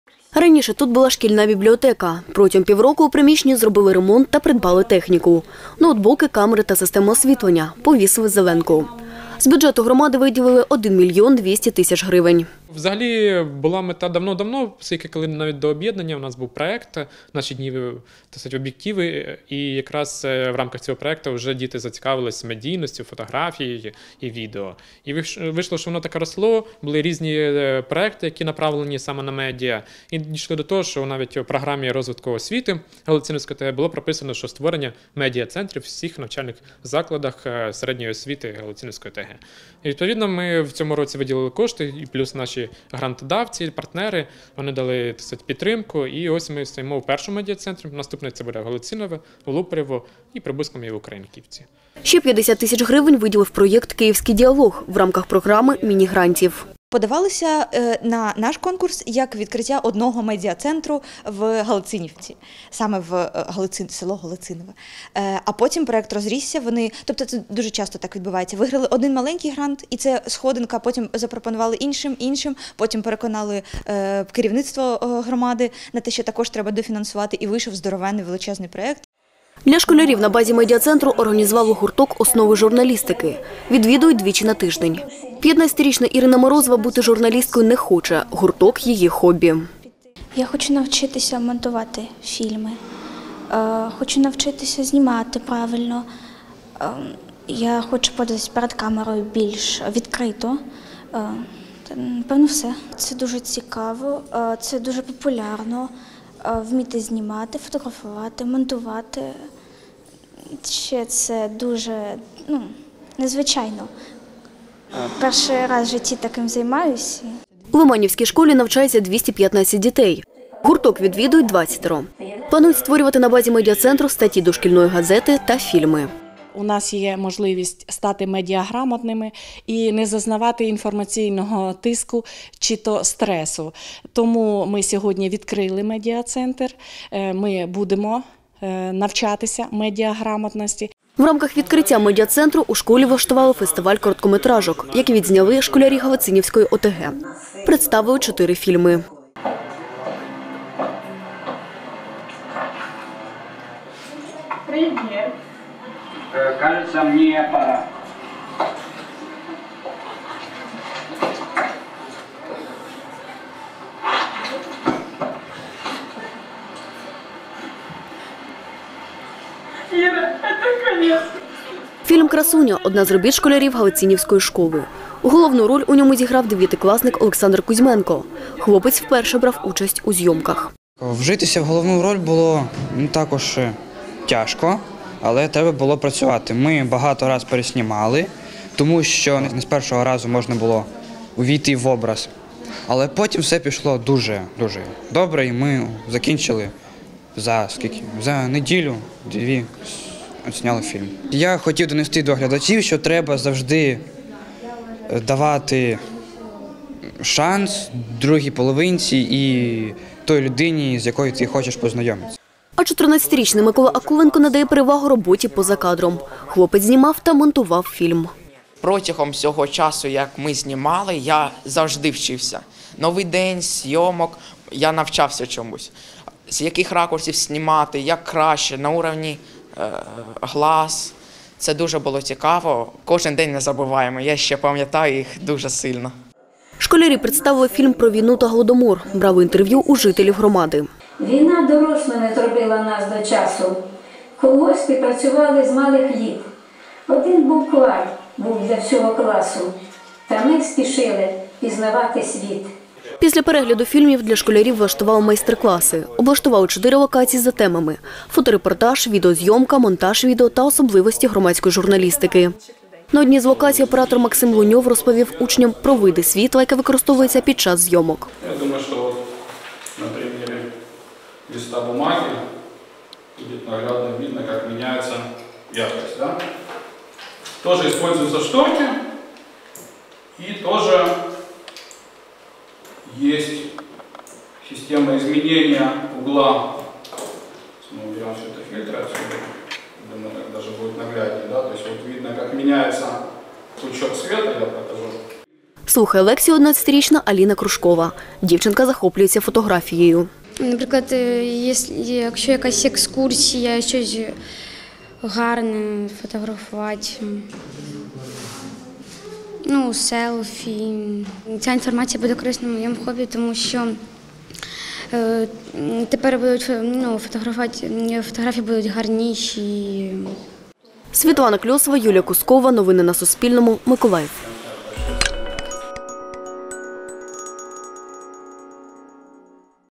Медіа-центр відкрили у Лиманівській школі
У Лиманівській школі  Галицинівської об’єднаної територіальної громади відкрили Медіа-центр NOVA.  Це перший такий медіа-простір у громаді. Школярі навчаються тут основам журналістської та операторської майстерності.